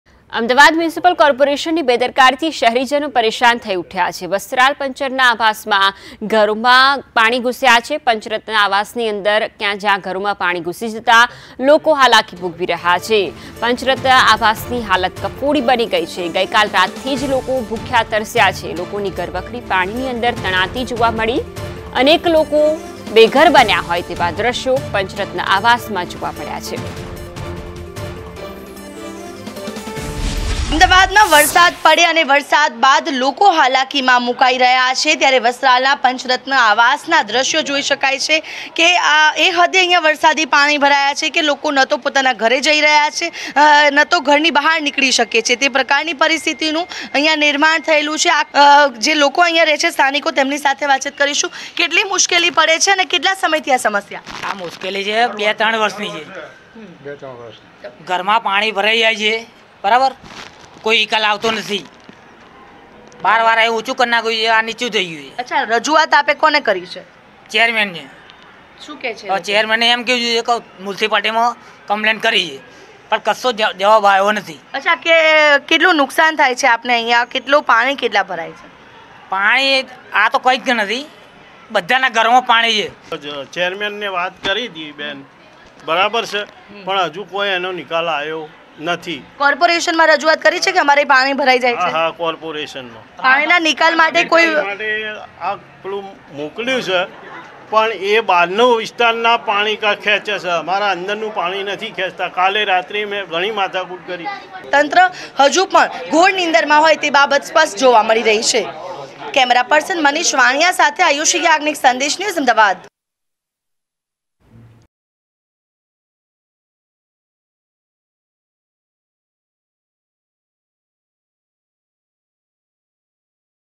Weather Update | AMCની બેદરકારીથી શહેરીજનો પરેશાન | Rain | Monsoon
AMCની બેદરકારીથી શહેરીજનો પરેશાન
વસ્ત્રાલ પંચરત્ન આવાસમાં ઘરોમાં ઘૂસ્યા પાણી
જુઓ પંચરત્ન આવાસની કફોડી હાલત
ગઈકાલ રાતથી લોકો ભૂખ્યા અને તરસ્યા
લોકોની ઘર વખરી પાણીમાં તણાઈ